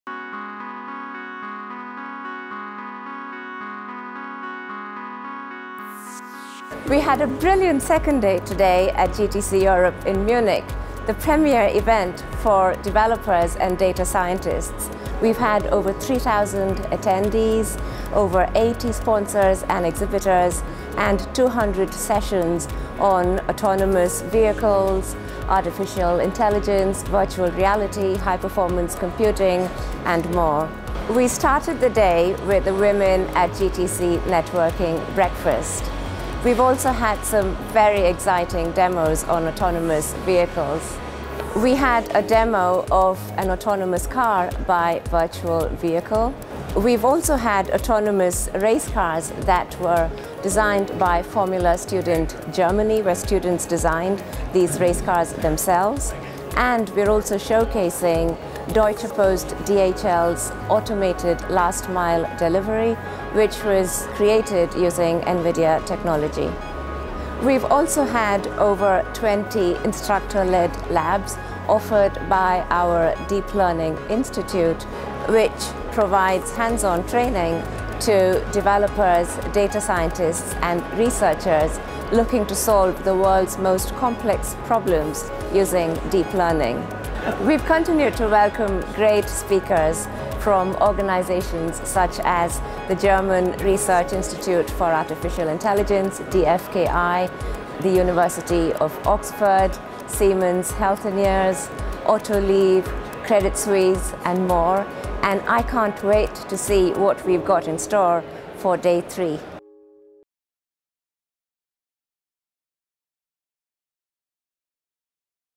GTC Europe 2017 - Day 2 Wrap-Up
Second Day wrap-up of NVIDIA's GPU Technology Conference Europe in Munich, October 10 - 12.